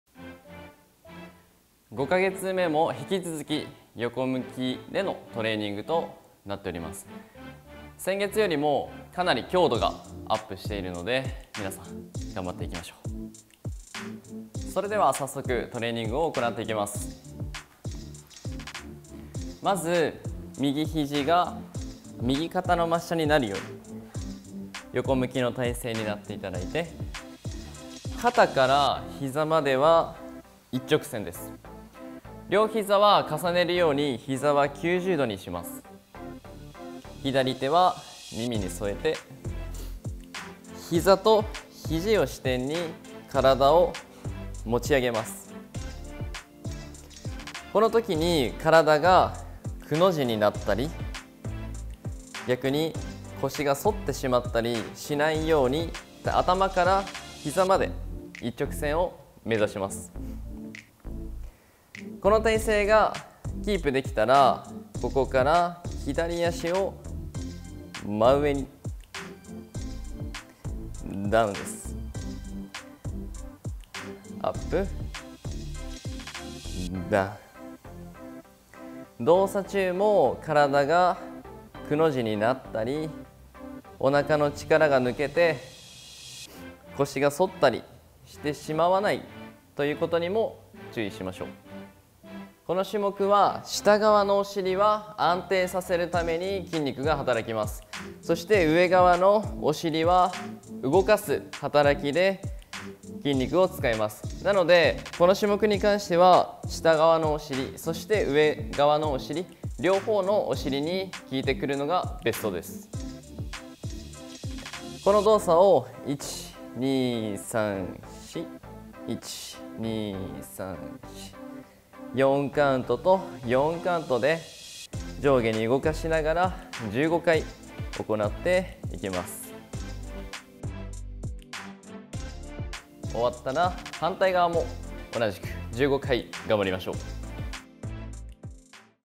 お尻のトレーニング⑤ サイドプランク with ヒップアブダクション
お尻は人間の体の中心にあり、最も大きな筋肉が備わっています。鍛えることで筋肉量が増え、代謝の向上や下半身の安定につながるなどさまざまなメリットがあります。

　これから１２カ月かけて、お尻のトレーニングに取り組みます。人間の発育段階に沿って、あお向け、寝返り、四つんばい、つかまり立ちのような順番で、お尻を重点的に鍛えます。体の大きな土台となる筋肉を鍛え、日常生活において疲れにくく、人生１００年時代を見据えた長く動ける体をつくっていきます。

講師は大分市のパーソナルトレーニングスタジオ「ヴェンティトレ」代表の武生瞬さんです。